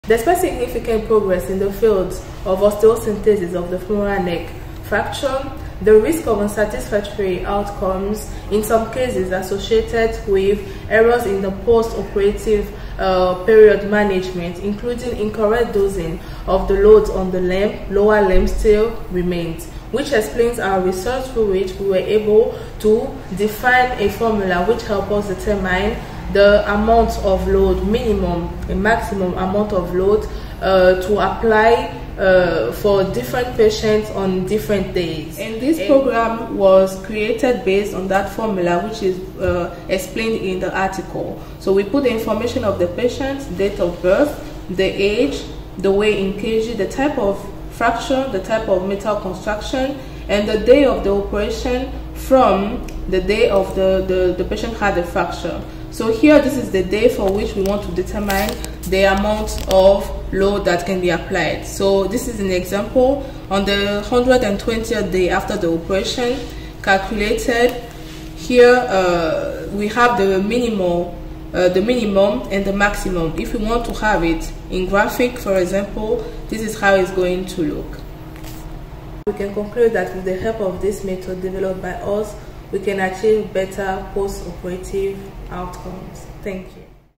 Calculation and application of a safe range of load in patients after osteosynthesis of femoral neck
Patients can have a better postoperative outcome owing to the possibility of determining the optimal range of load on the operated limb, which makes it possible to draw up an individual plan for dosing the load from the first day of the postoperative period to the onset of consolidation.